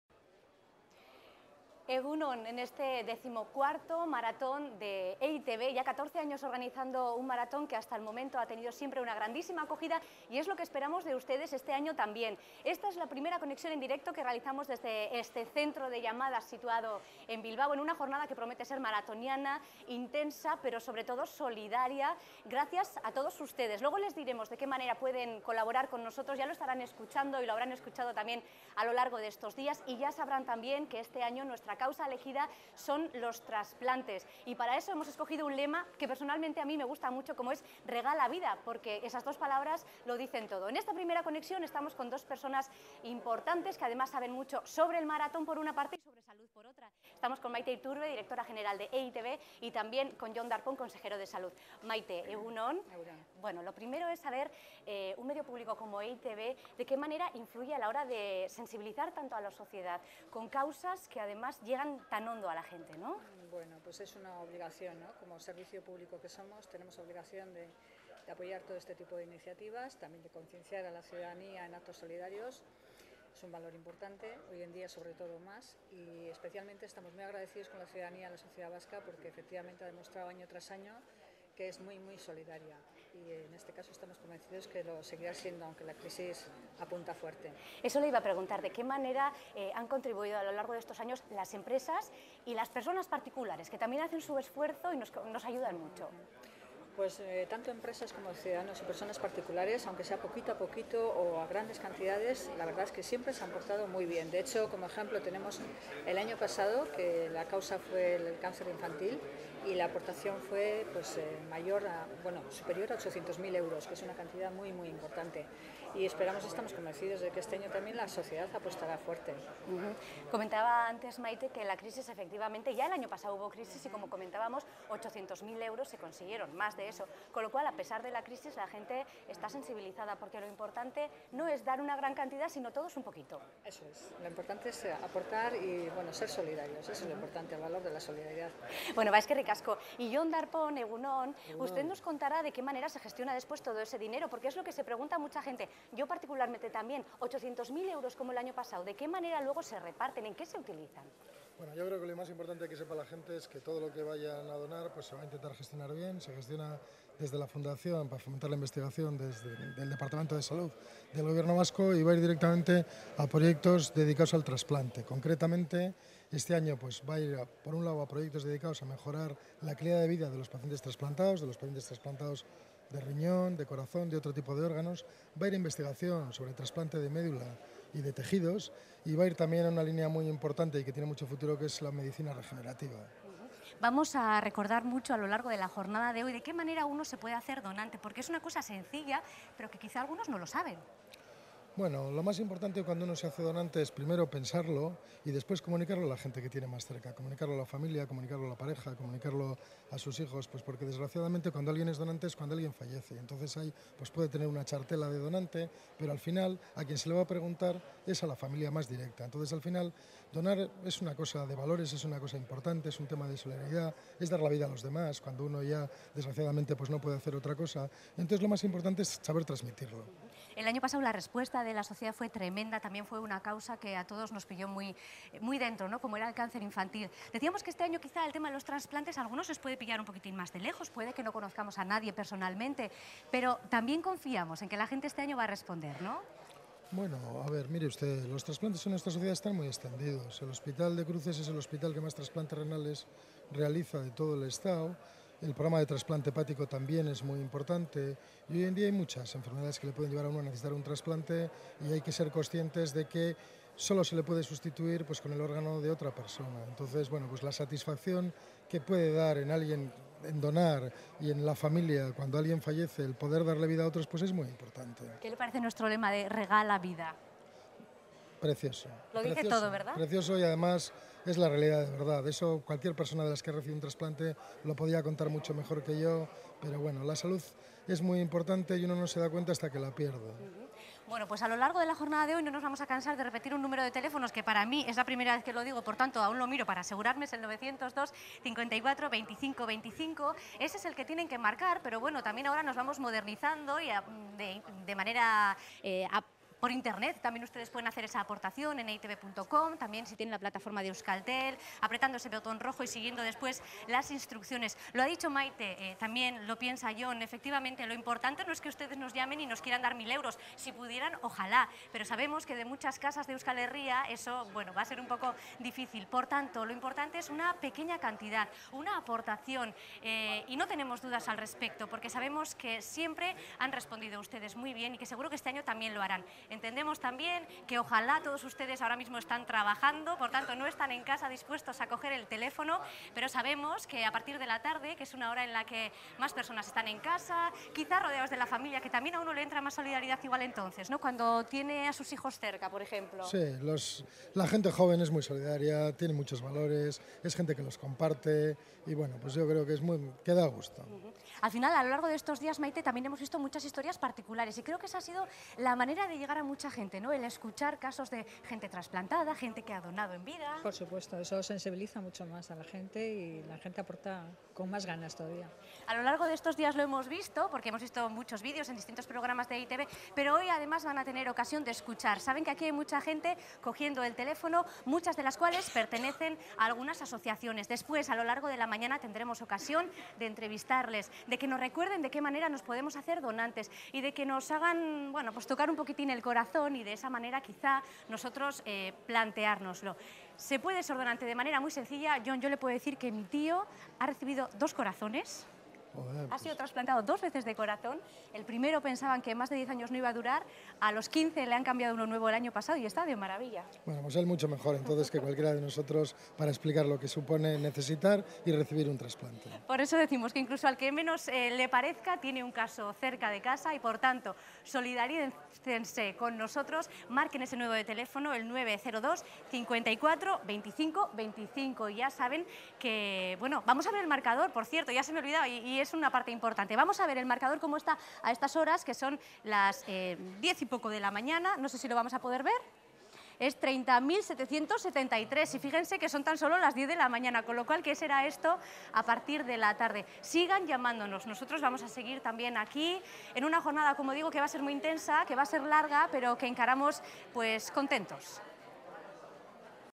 Ya está en marcha el maratón solidario de EITB
Maite Iturbe y Jon Darpón han sido los encargados de dar comienzo al maratón solidario de EITB de este año.